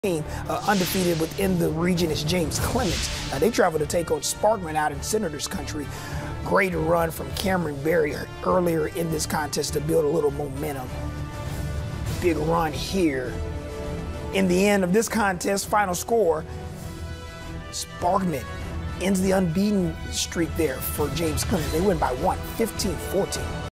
48 Blitz Week 7: James Clemens vs. Sparkman
The Senators squeezed past the Jets for the 15-14 win.